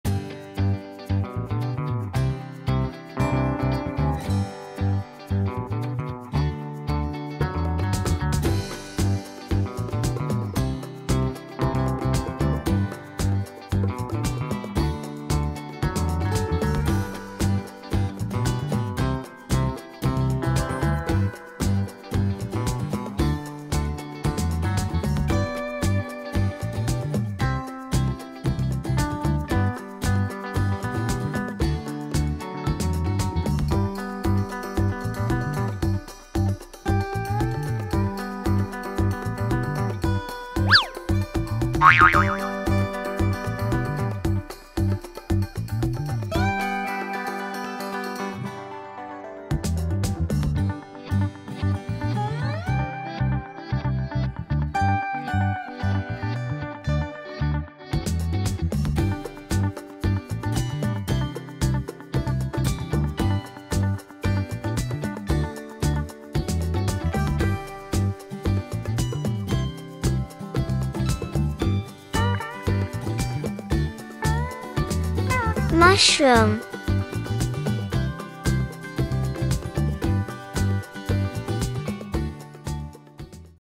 Drawing and coloring mushroom for kids easy and step by step? 😍🌹
In this video learn how to draw and color a mushroom for kids easily and step by step. In this coloring page kids can get familiarized with easy drawing and coloring as well as different kinds of colors and their correct pronunciations.
Welcome to "Rainbow Kids Shining" coloring page. 🌹🙂